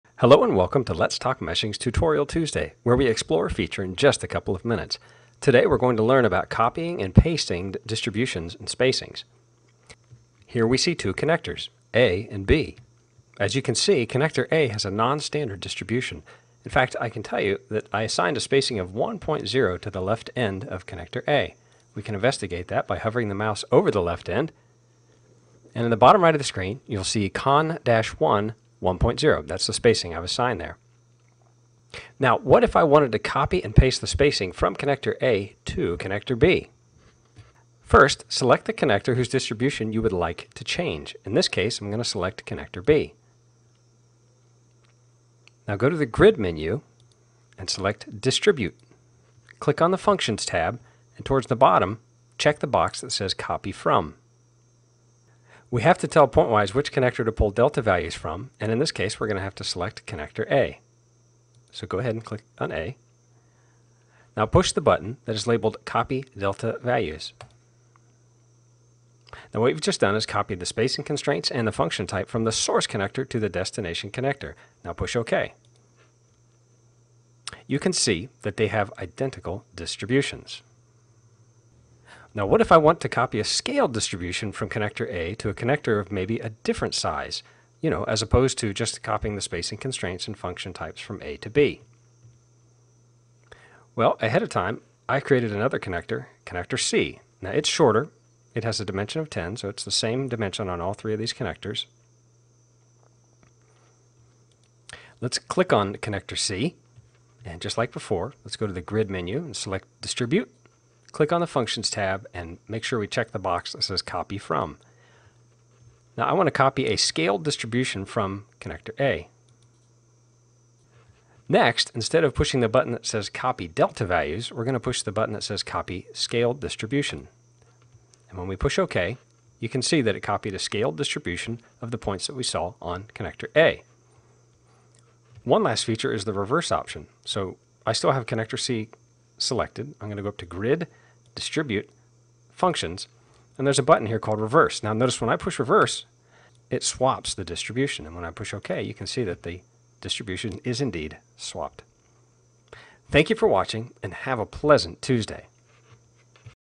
Copy and Paste Connector Distributions
In this video we demonstrate how to copy and paste connector distributions from one connector to another using the Grid, Distribute command in Pointwise.

A connector is a grid curve that has a dimension, spacing constraints, and a distribution function. The distribution function enforces the distribution of the grid points along the connector. Both the spacing constraints and the distribution function can be copied from one connector to another using the Grid, Distribute command.